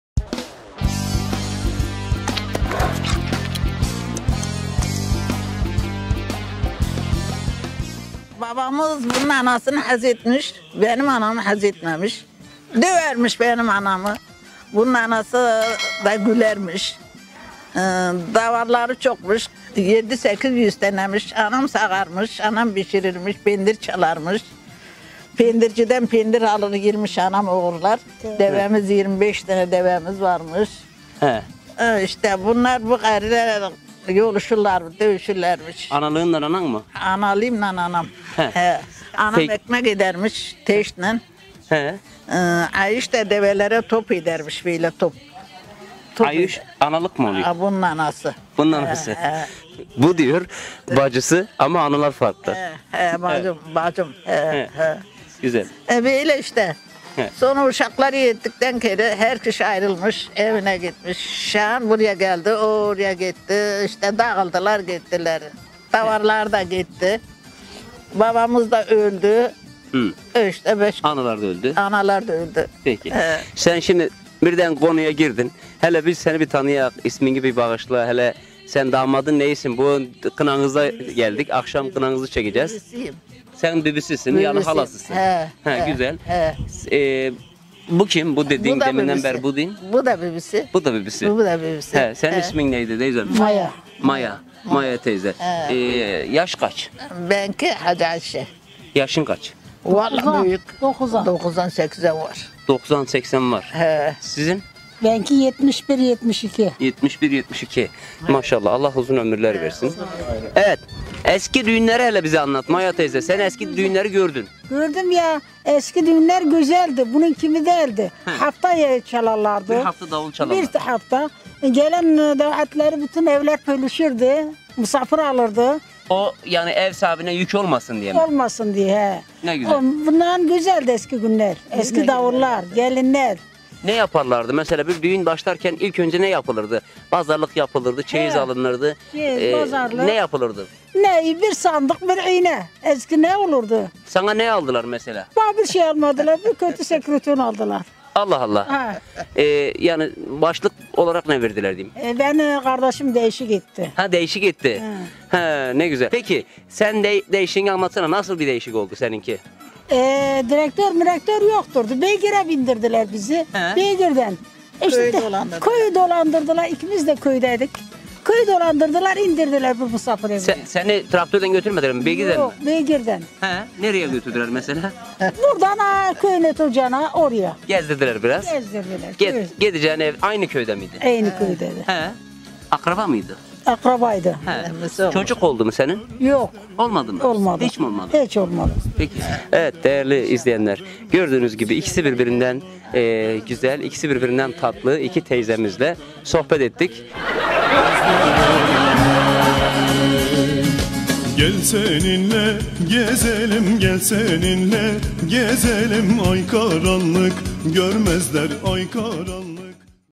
Babamız bunun anasını haz etmiş, benim anamı haz etmemiş
Havahüyük köyünde 2 bacı ile yapmış olduğumuz komik sohbet😊.

Merhaba Gönül dostlarım; Ben Adil ŞAN.
Suriye Türkmenleri ile alakalı tüm videoları kanalımdan takip edebilirsiniz.